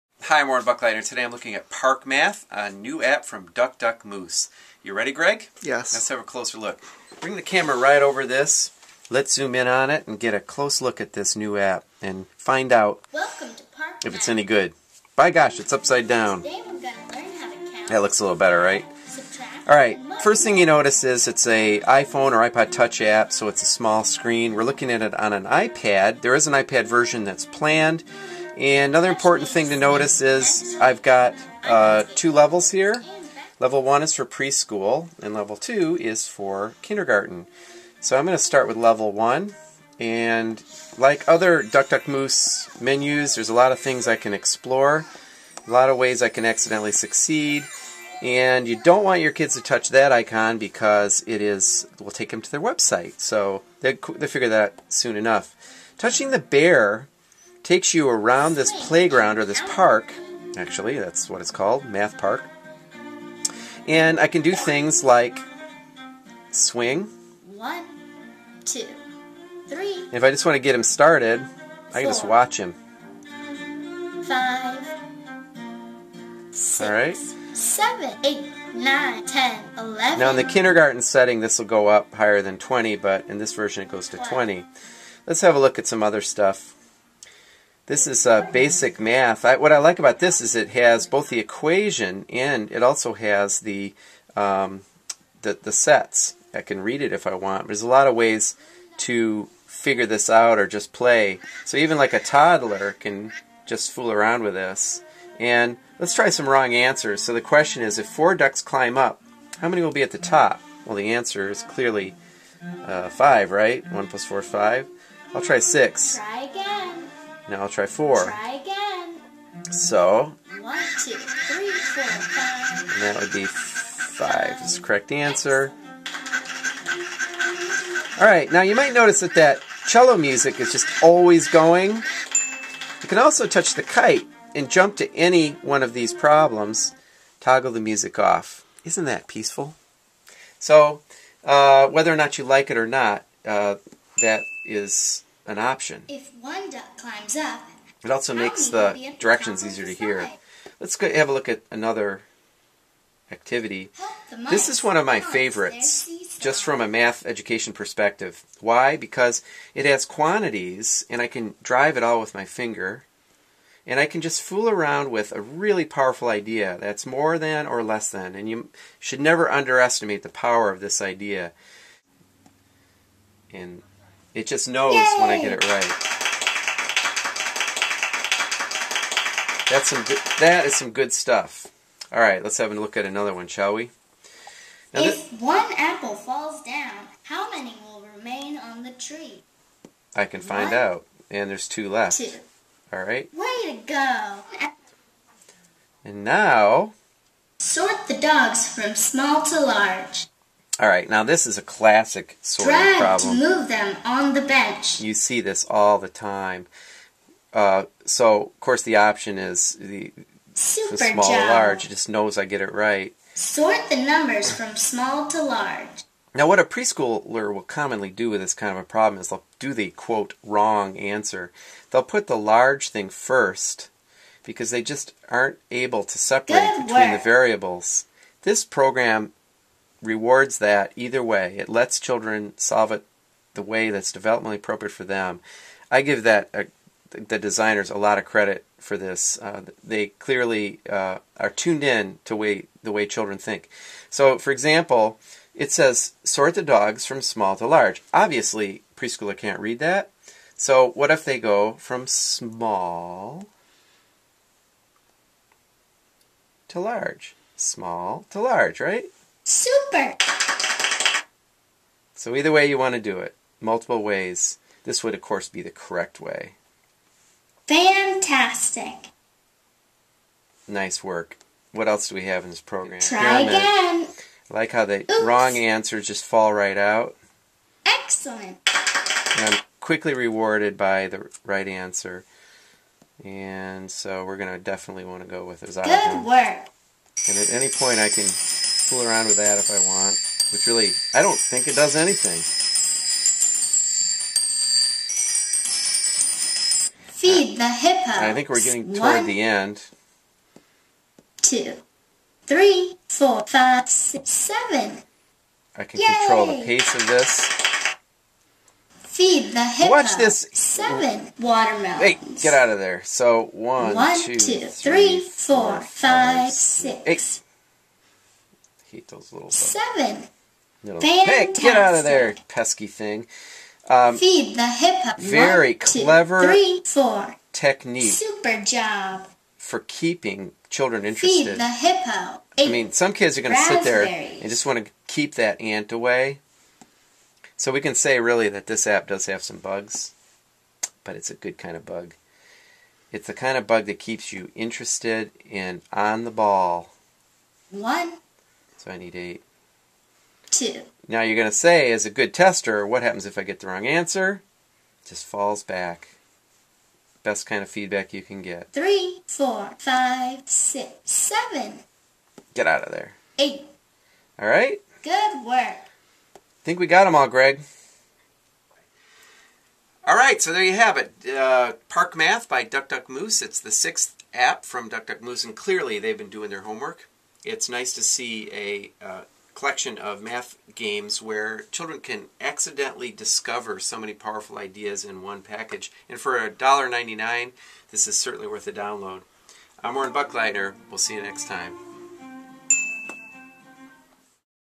[CTR Review] Park Math by Duck Duck Moose
This sixth App from Duck Duck Moose Design continues a streak of excellence in early childhood interactive craftsmanship that dates back to last year's Wheels on the Bus.  The App is designed for iPhone and iPod Touch and contains seven easy-to-explore games, each with multiple ways to try out some powerful math concepts -- of the variety that will someday loom at the root of an SAT question.
The games include Swing (Count up to 50 as a rabbit swings. You can either watch, or "push" the rabbit with your fingertip); Slide (Help ducks climb to the top of a slide. Quantities are show both visually and in an equation, simultaneously); Seesaw (Balance a seesaw by adding and subtracting mice); Apple Tree (Subtract as apples fall from a tree); Sandbox (Complete patterns by dragging and dropping toys); Bench (put dogs and numerals in order, from smaller to larger. It's especially nice that you can make the order from either left to right or right to left); and Picnic: Counting - Feed a hippo the correct number of food items.
It is possible to toggle between two levels of play (preschool and kindergarten). Level 1 includes counting up to 20 and addition/subtraction with numbers up to 5. Level 2 includes counting up to 50 and addition/subtraction with numbers up to 10. The iPad version, called Park Math HD is in the works, and and will include a third level for First Grade with counting up to 100 and addition/subtraction with numbers up to 20. This is a 13.5 MB download, for  iPhone OS 3.0 or later. It is well worth the $2 download.  See the full review, with ratings, in the October issue of Children's Technology Review.